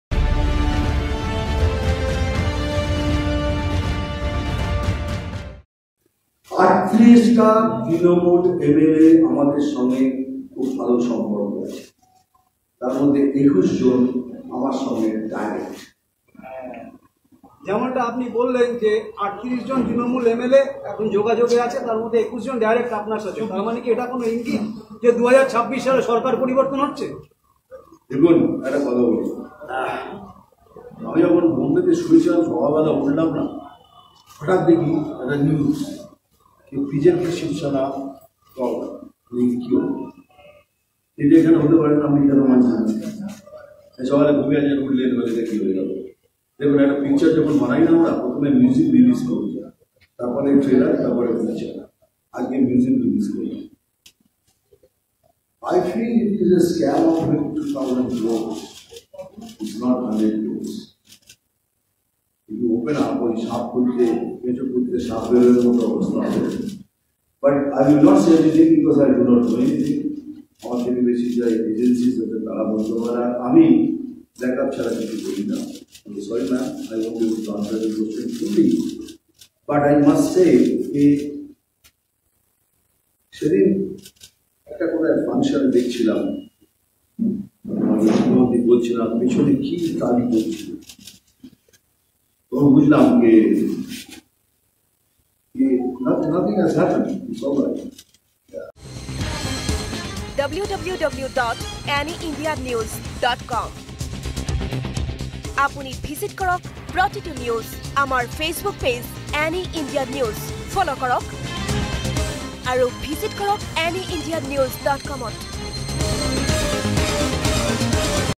West Bengalত ৰাজনৈতিক পৰিবৰ্তনৰ ইংগিত Mithun Chakroborthyৰ | Bollywood | Ne India News
পশ্চিম বংগত ৰাজনৈতিক পট পৰিবৰ্তনৰ আজি ইংগীত দিলে অভিনেতা তথা বিজেপি নেতা মিঠুন চক্ৰৱৰ্তীয়ে ।
আজি পশ্চিম বংগ বিজেপিৰ কাৰ্যলয়ত অনুষ্ঠিত এক সংবাদমেলত মিঠুন চক্ৰৱৰ্তীয়ে কয় -" মই মুম্বাইত আছিলো । নিশা শুই উঠি হঠাৎ দেখিলো বিজেপি আৰু শিৱসেনাৰ চৰকাৰ গঠন হল মহাৰাষ্ট্ৰত । এয়া কি হল , এয়া দেখোন পশ্চিম বংগতো সম্ভৱ । মহাৰাষ্ট্ৰৰ দৰে ৰাজনৈতিক পট পৰিবৰ্তন পশ্চিম বংগত হব নোৱাৰে ,এই কথাষাৰ মই মানিব লব নোৱাৰো । "
অভিনেতা মিঠুন চক্ৰৱৰ্তীৰ দাবী তৃণমুল কংগ্ৰেছৰ ৩৮ গৰাকী বিধায়কে বিজেপিৰে সম্পৰ্ক ৰক্ষা কৰি চলিছে ...